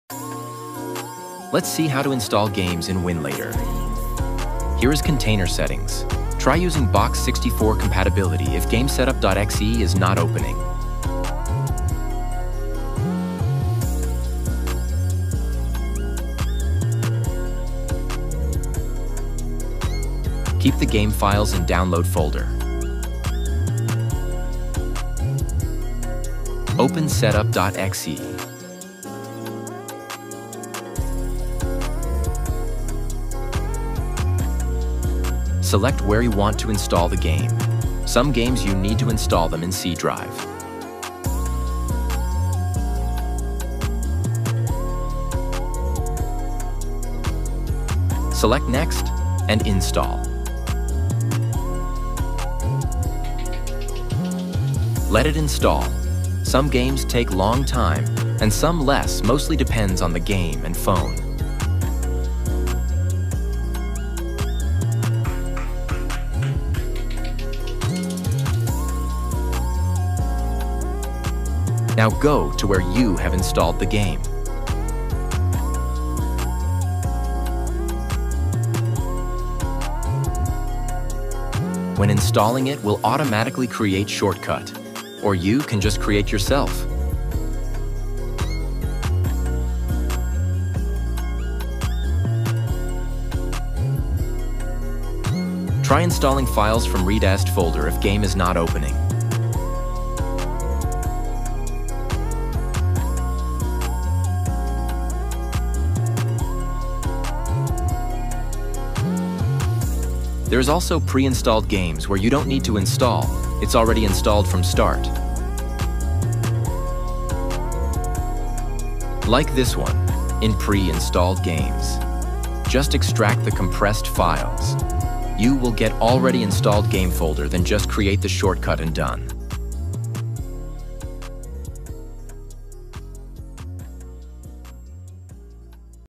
How to Install Games on Winlator Android
How to Install Games on Winlator (Windows Emulator) Version 9.0 Android
📱Device:
--POCO F6
--Snapdragon 8s Gen 3
--12 GB Ram // 512 GB Storage
-- No Root

(Music Credit)
THANKS TO DEVELOPERS
Winlator ‪@brunodev85‬
Box86/Box64 ‪@ptitSeb123‬
CHECK OUT